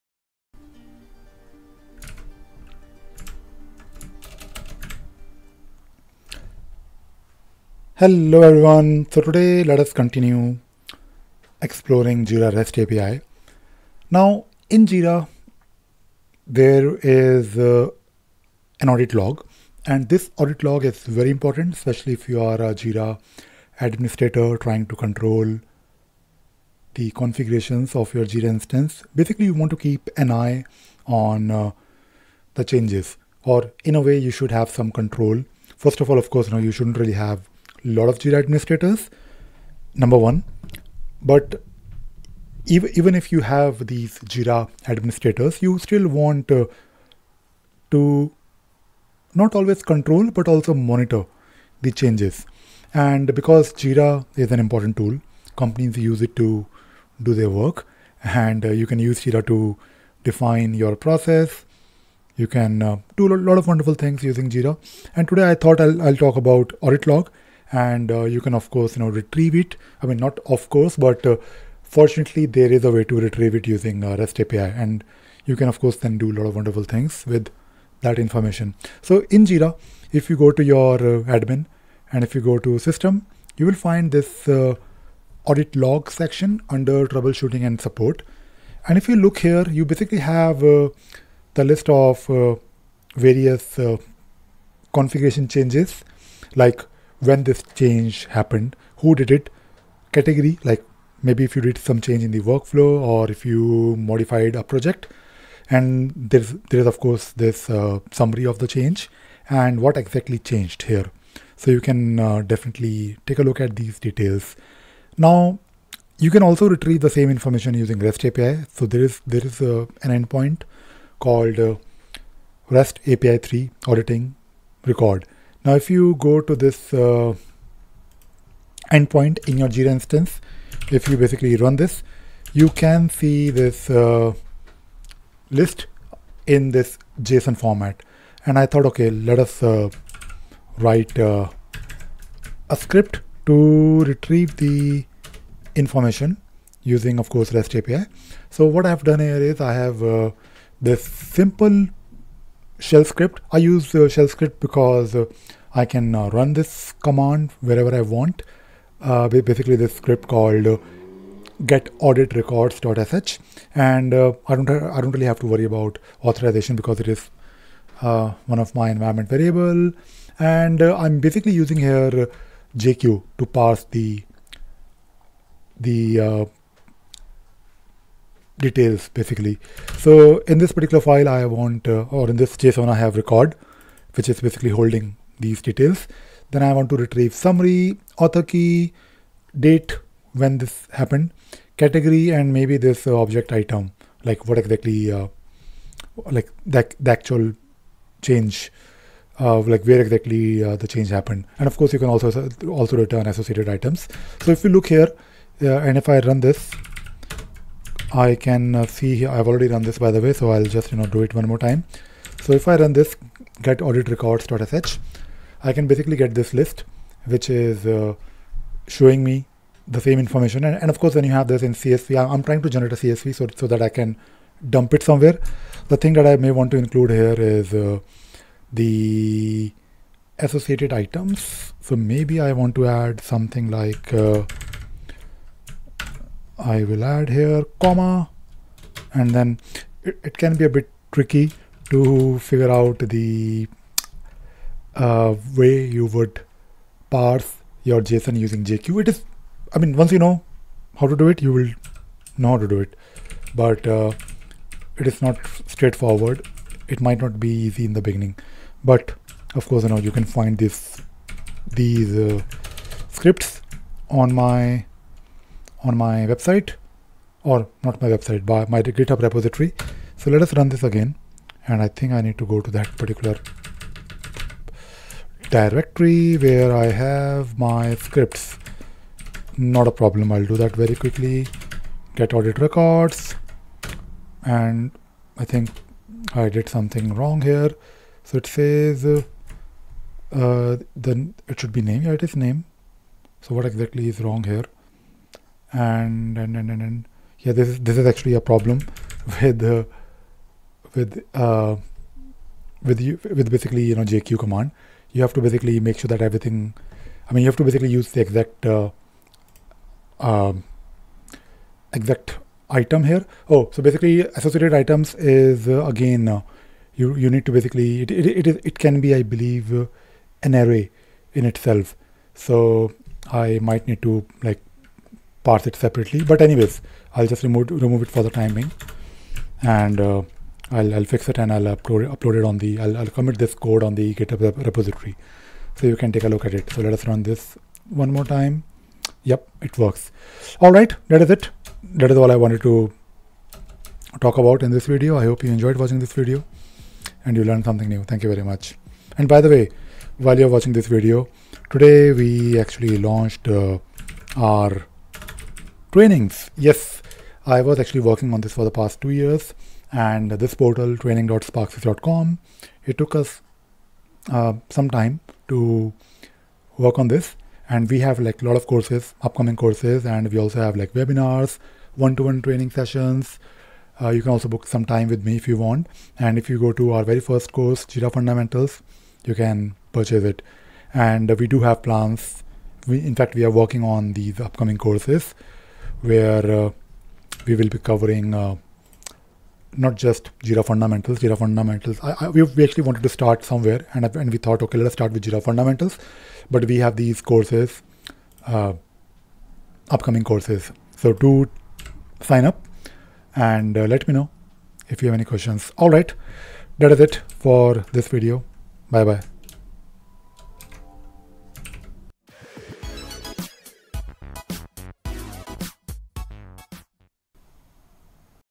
Jira REST API - Get Audit Records
It is extremely important to control who can do administrative work on your Jira instance and monitoring the configuration changes could be crucial. Fortunately there is an audit log which you can look at and also retrieve using the Jira REST API